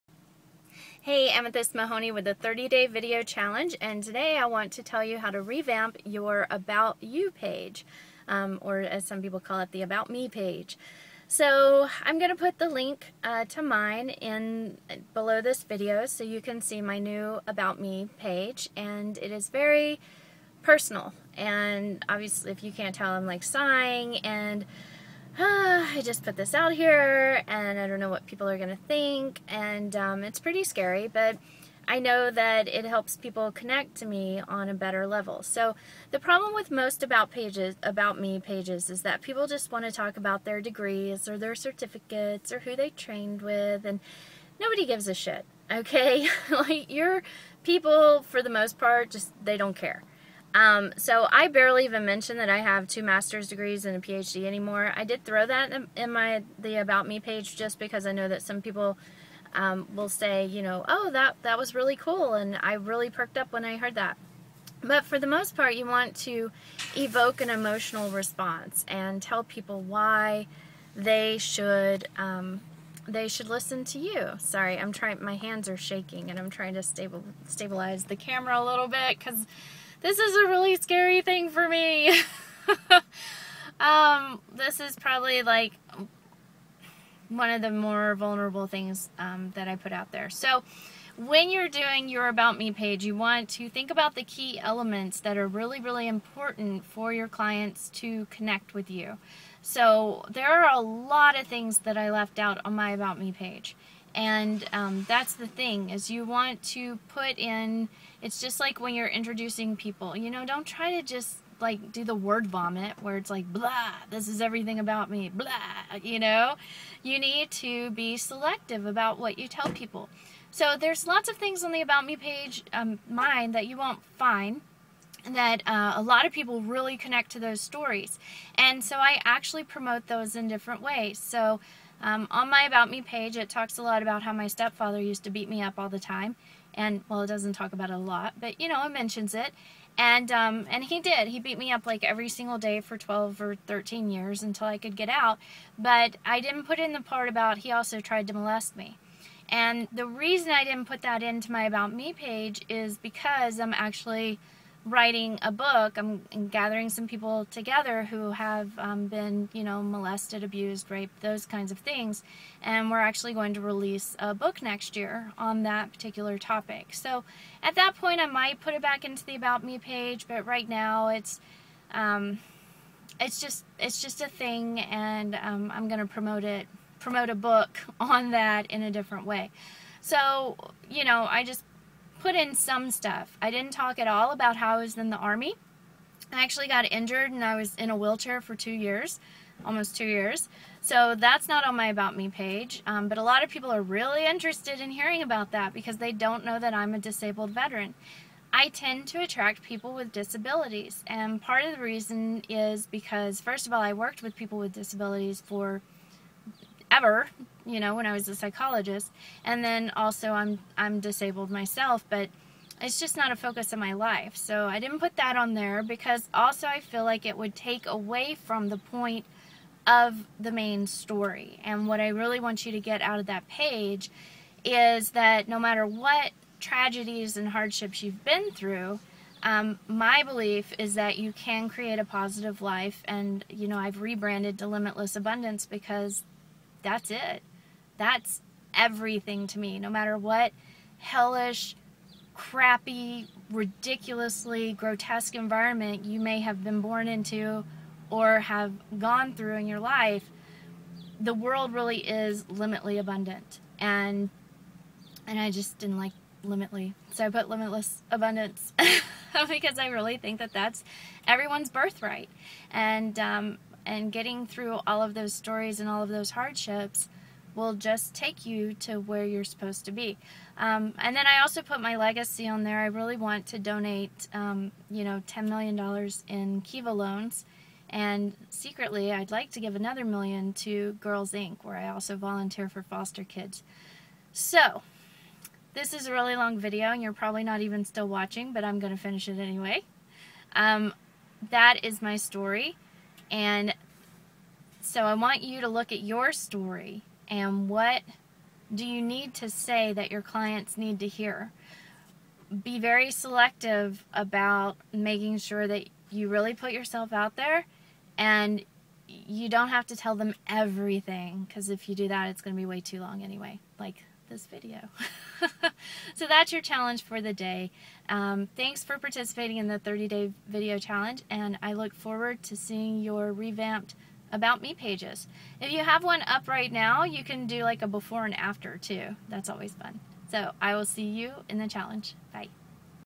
What's Your Story?
Do you use your About Me page to really share your story? Here's a video did about recently changing my story and really coming out about who I am